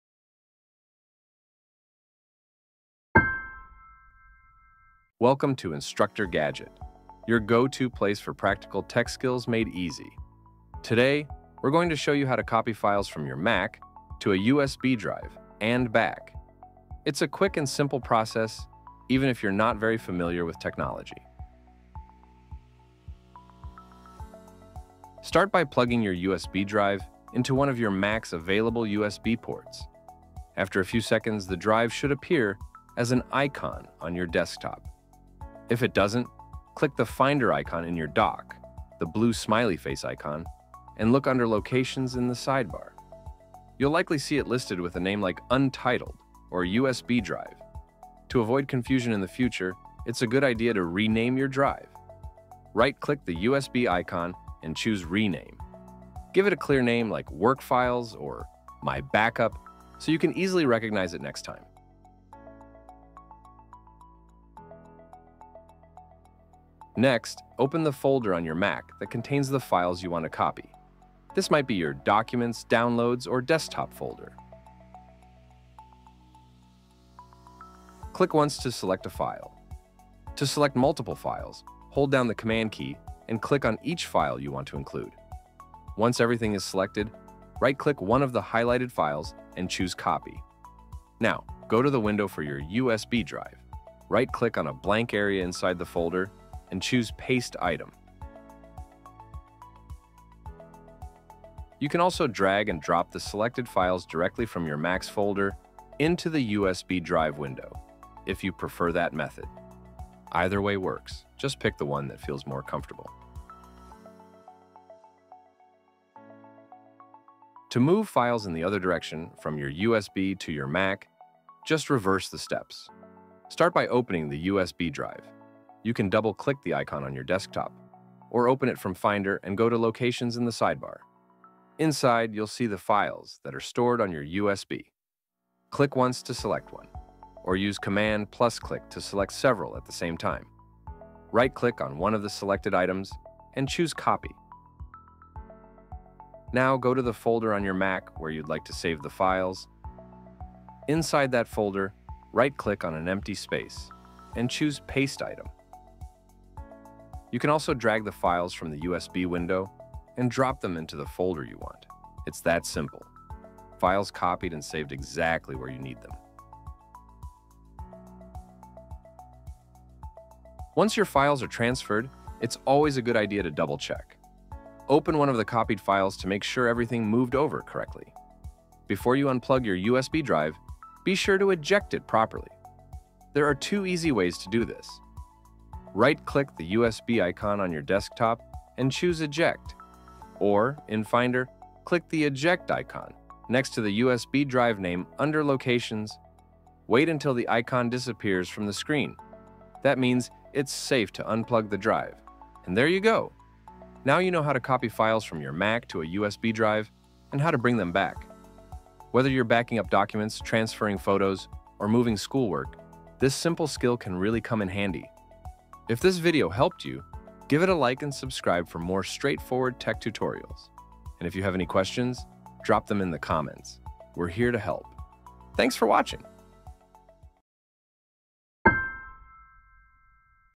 How to Copy Files To & From USB Drive on Mac (Step-by-Step for Beginners)
Need to move files from your Mac to a USB stick—or the other way around? Whether you’re backing up documents, transferring photos, or sharing files, this quick tutorial will show you exactly how to copy files between your Mac and a USB drive without confusion.

In this video, you’ll learn:
✅ How to locate your USB on a Mac
✅ How to rename your USB drive
✅ How to copy files to and from your USB
✅ How to safely eject your USB to avoid data loss

This guide is perfect for beginners or anyone who just wants a simple, reliable method to transfer files.
🎯 No tech experience needed—just follow along!

💬 Questions? Drop them in the comments. We’re here to help!